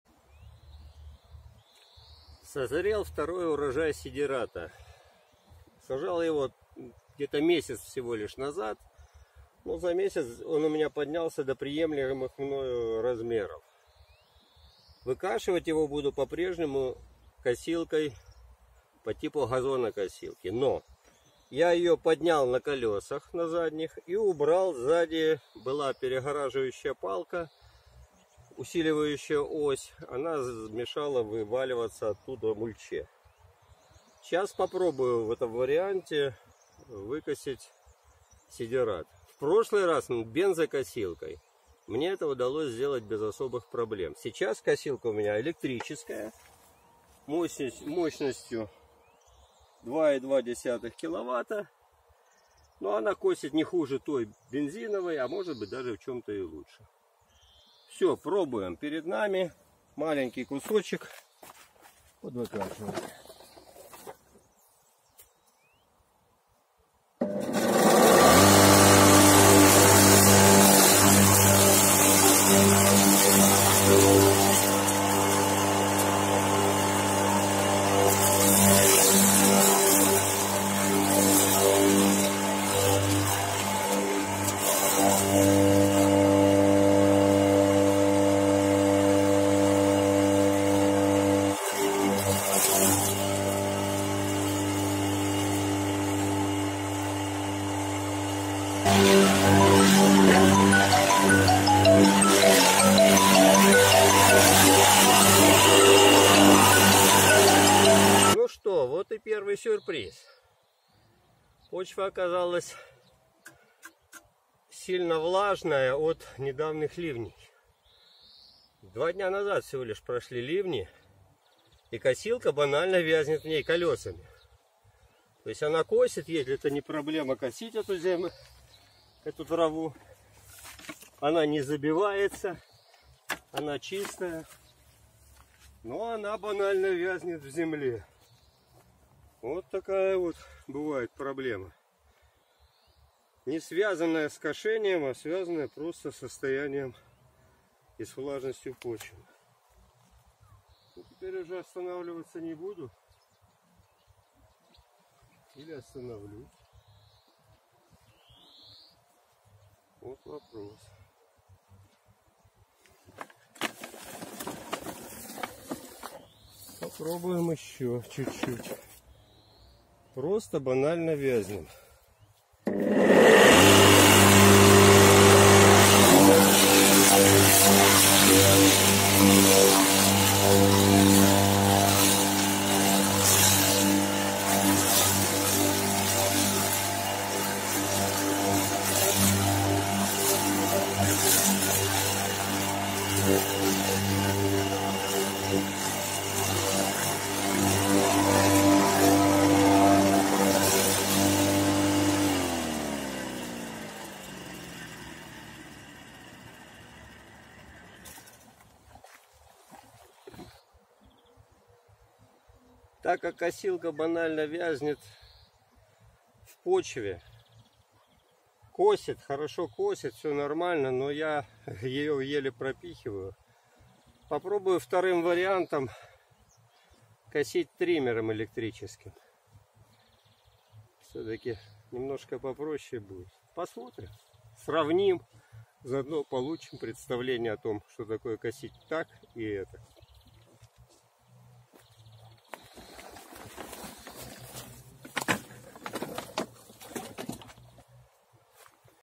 Косим сидерат под перепашку // Электрокосилка Л@плош
Почва после бури, да по сидерату... Пух! Проваливаемся!